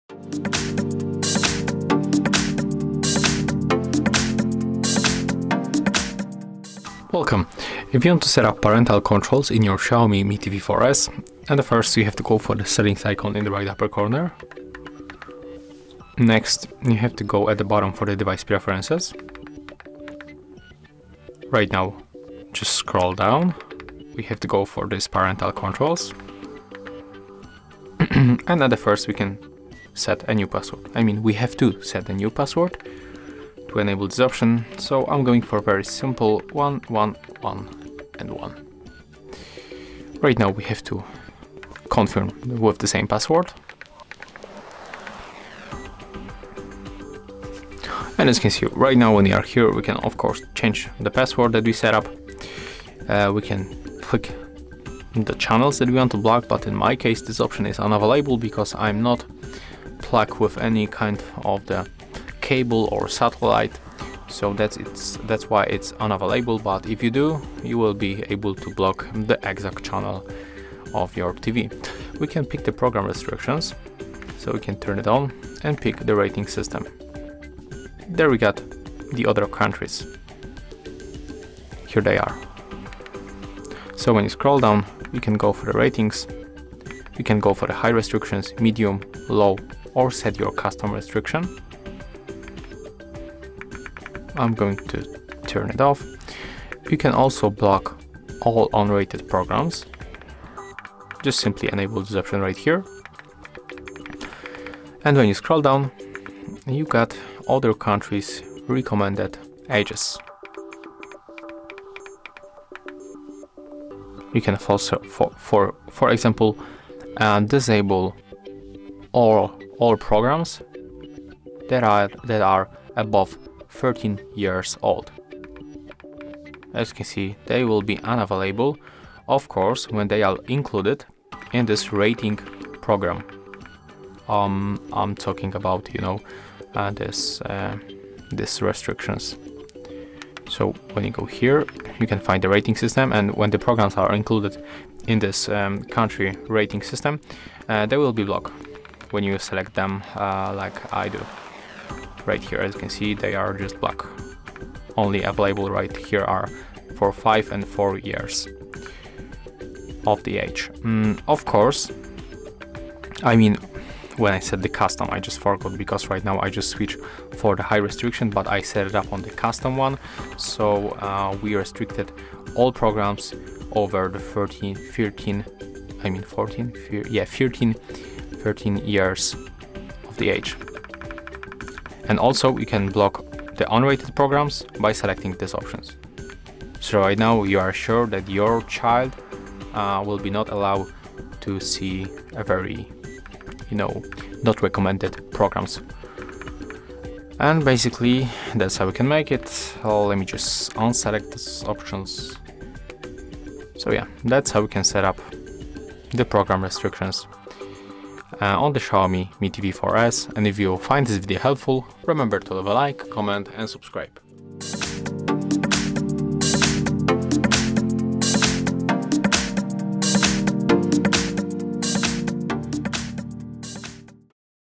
How to Set Up Parental Controls in Xiaomi Mi TV 4S?
Parental Controls is an option where you can block access to content on your TV.

How to Manage Parental Controls in Xiaomi Mi TV 4S? How to Use Parental Controls in Xiaomi Mi TV 4S?